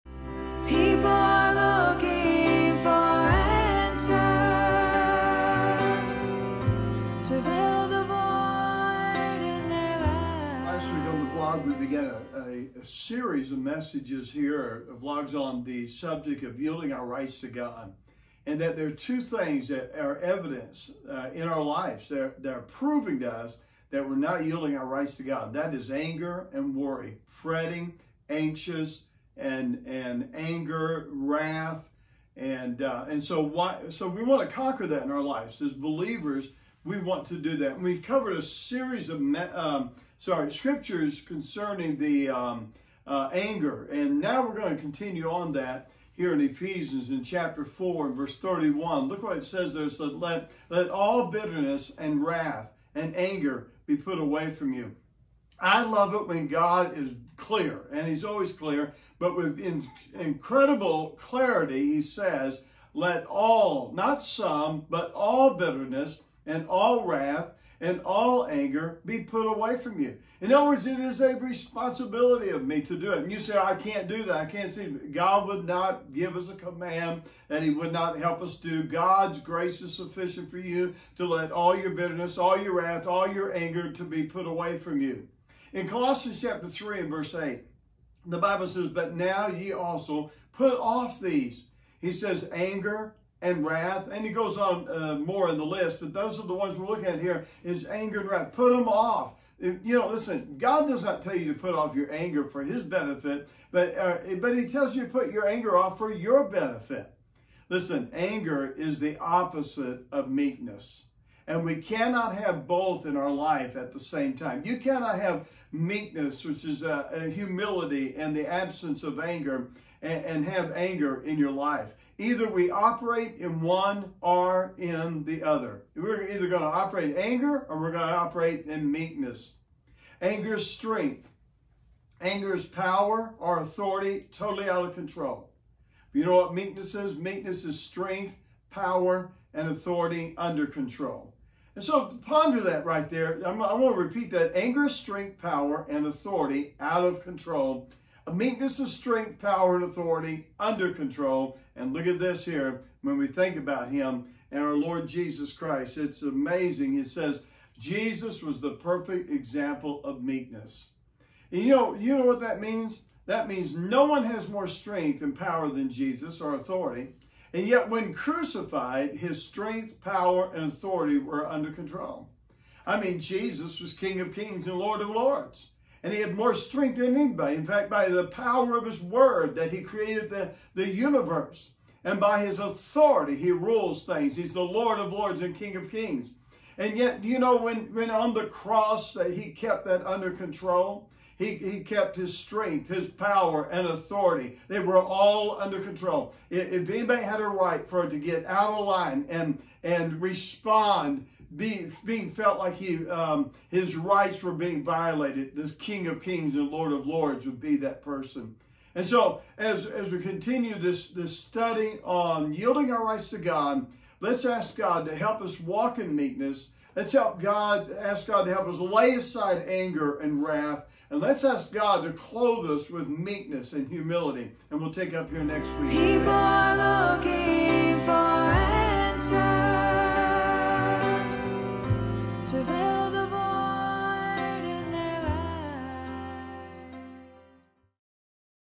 Yielding Our Rights To God Part 2
Anger and Worry are Evidences that we are not Yielding our Rights To God
Ephesians 4:31 - Let all bitterness, and wrath, and anger, and clamour, and evil speaking, be put away from you, with all malice.
The opposite anger is meekness. As Christians we must learn to put off anger and put on meekness
God would not tell us to put away something that He would not empower us to completely put away.